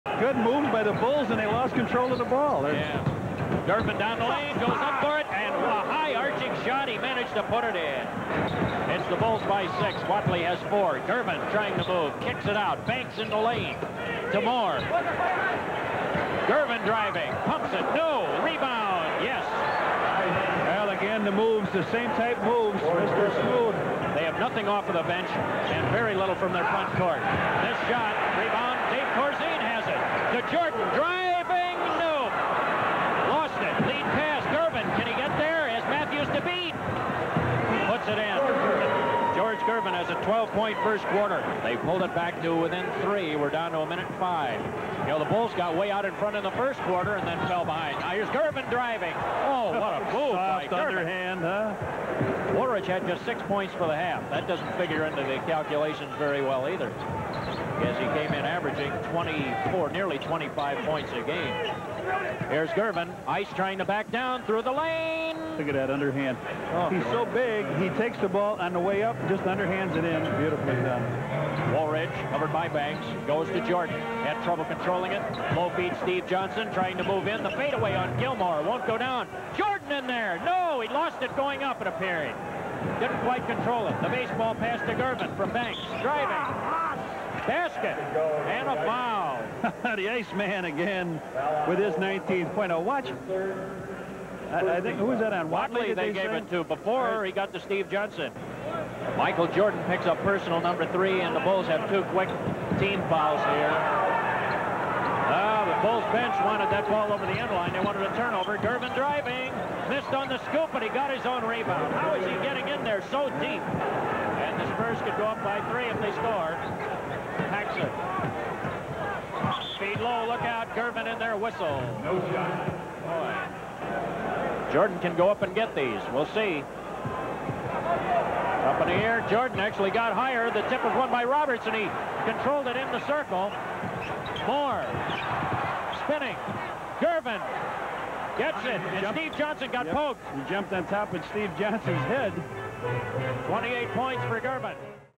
George Gervin Spams the Finger Roll Against MJ & Co.
November 13, 1984 - San Antonio Spurs legend George Gervin eased his way to 28 points in a 120-117 loss to the Chicago Bulls early in the 1984-85 regular season. Frequently matching up with rookie Michael Jordan, Gervin spent a significant portion of his evening utilizing his signature finger roll over the out-stretched arms of the Chicago defense.

A 12-time all-star, Gervin led the league in scoring four times in five years from 1978 to 1982. Including his four seasons in the ABA, Gervin finished his career with over 26,000 points, 17th all-time (as of 2021) among NBA/ABA contributors.